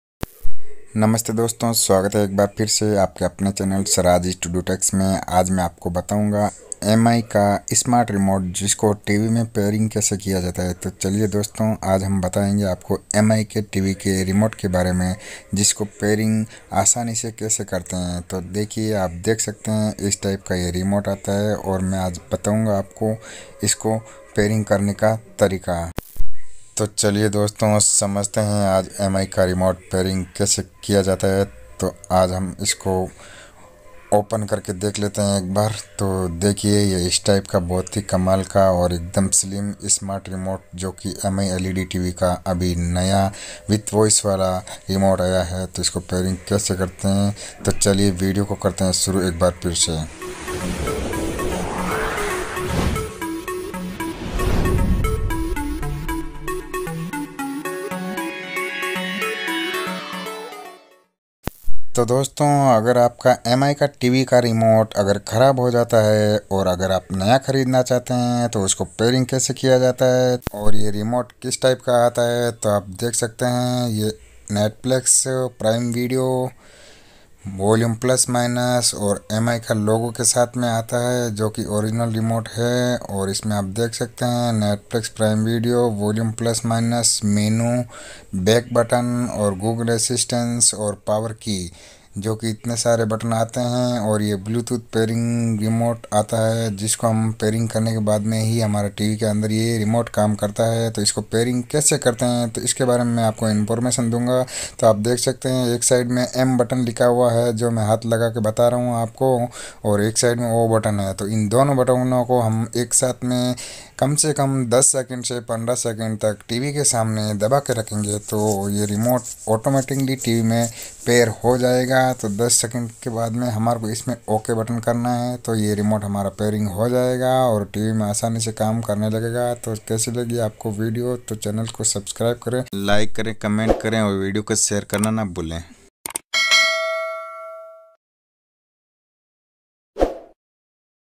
How To Pair New Mi Bluetooth Voice Remote ! Mi Remote Unboxing and pairing guides
हैलो दोस्तों- इस वीडियो मे मैने आपको mi smart tv के रिमोट को tv मे पैर कैसे करते है और उसको unboxing करके बताया है!